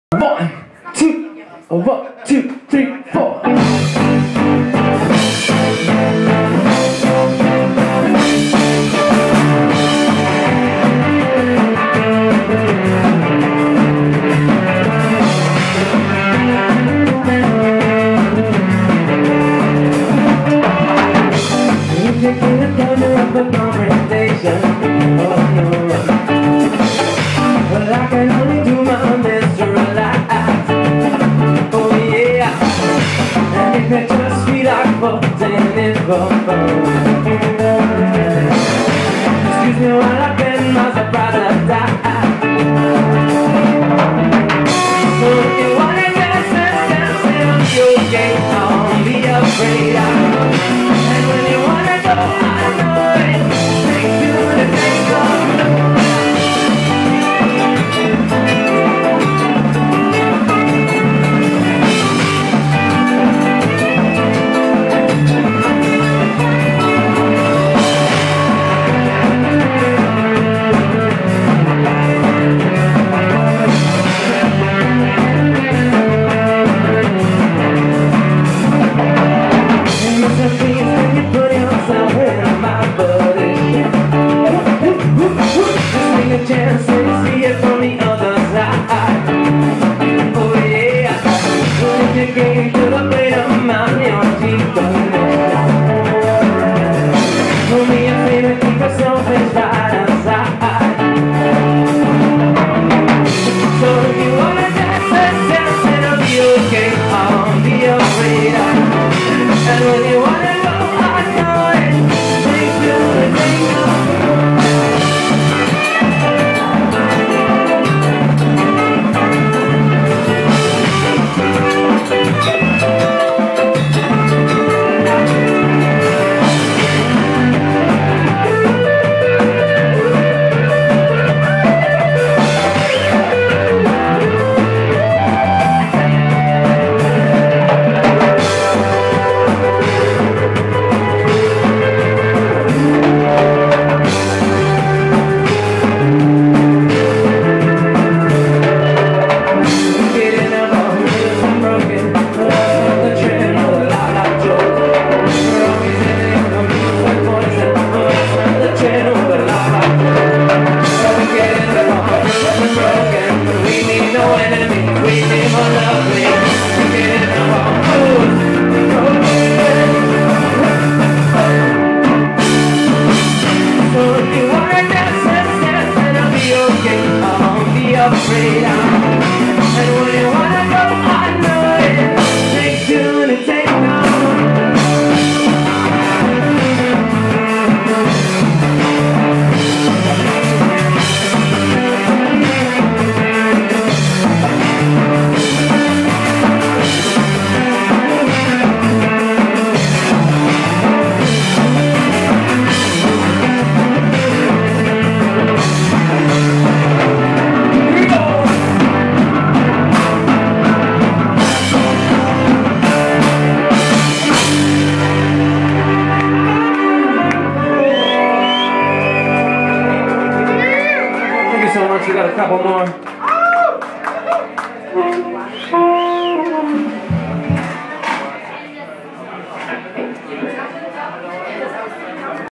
The Originalites - (Live at Tower Bar San Diego, CA)
Enjoy this live video of The Originalites rocking the roof off Tower Bar in San Diego, CA in 2014!